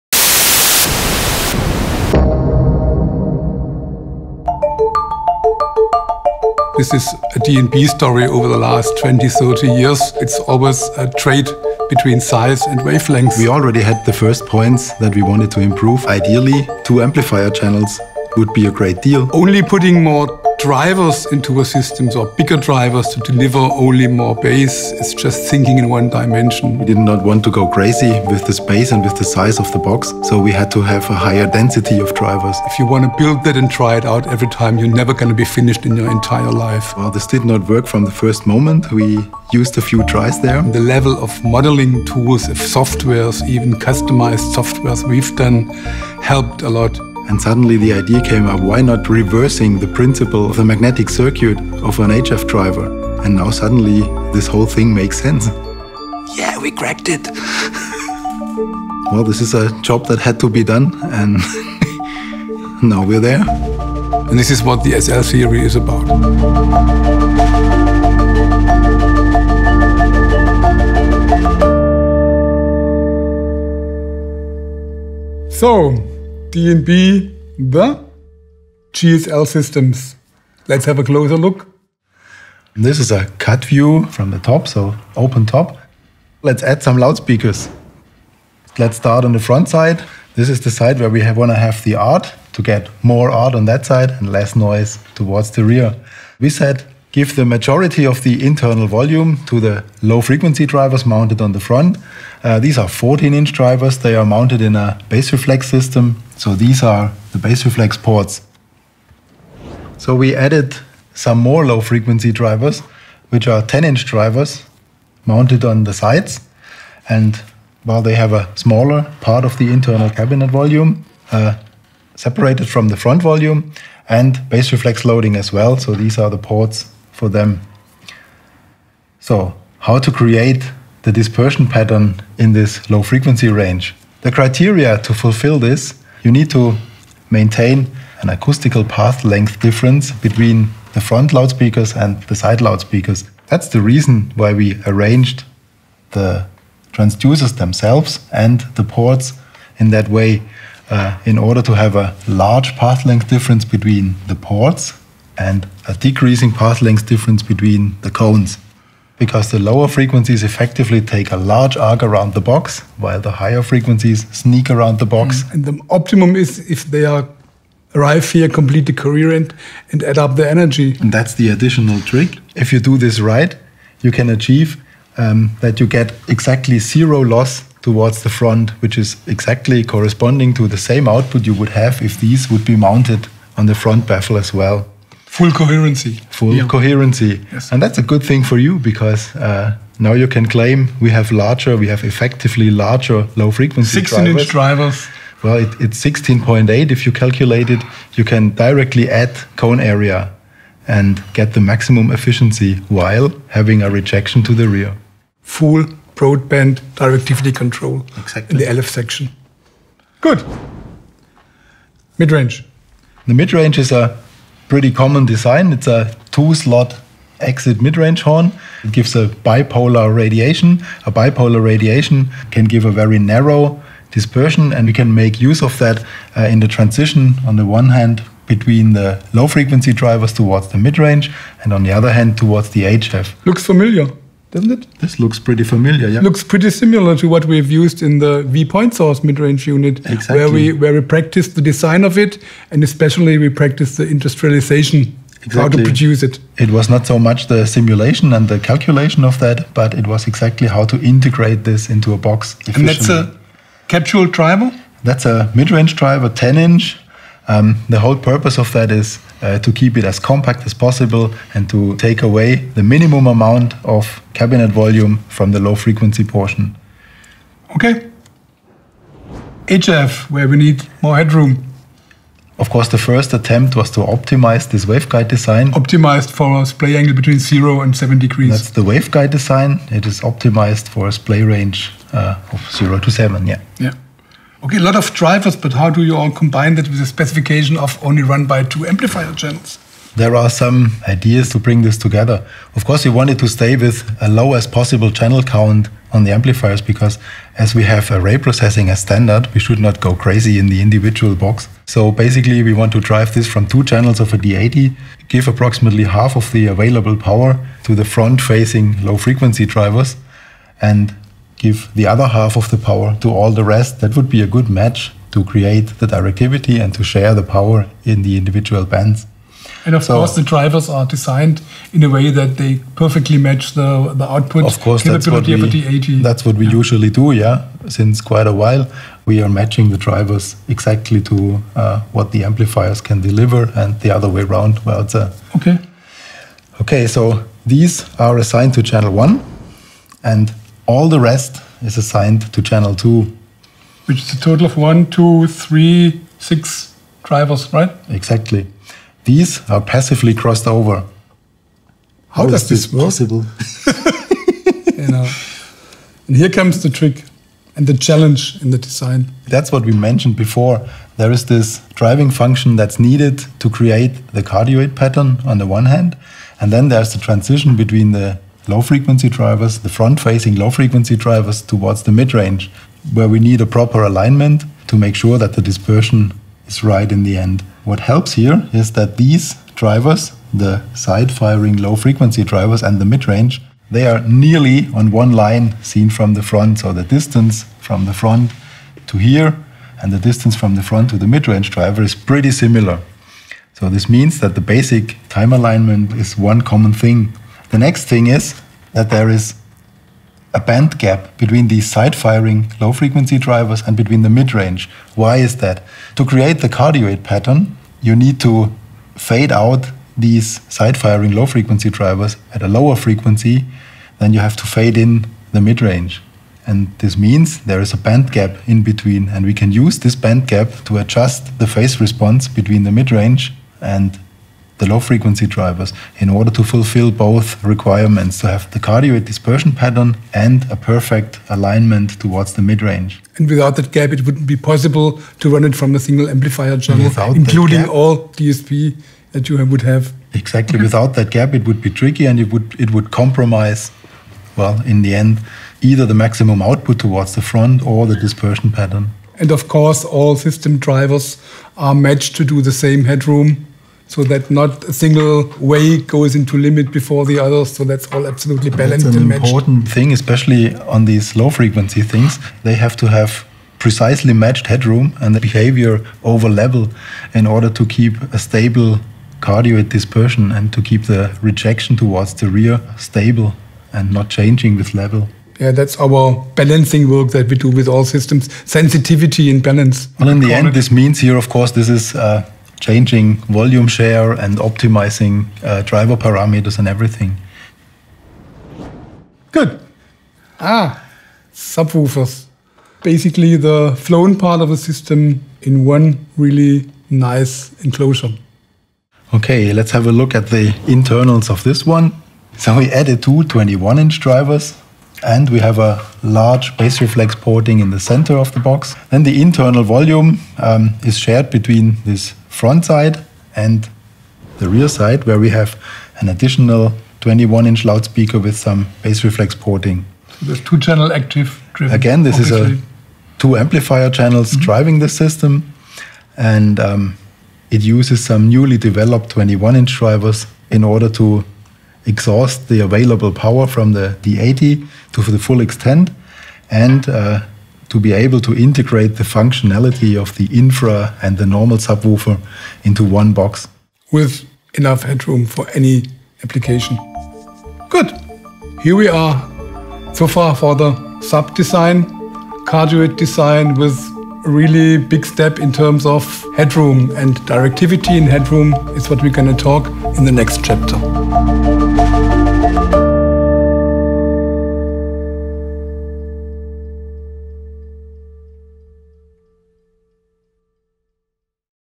SL-Series. Inside the box. A digital table talk
Discover the dream behind the new d&b SL-Series and how the team cracked the quest to build the ultimate large scale line array. Vier and Matthias introduce the vision to achieve the highest degree of horizontal dispersion control over the entire audio bandwidth. An illustrated cut view explores inside the cabinets to uncover the tricks and challenges that shaped the design. What does all this mean? “More art. Less noise.”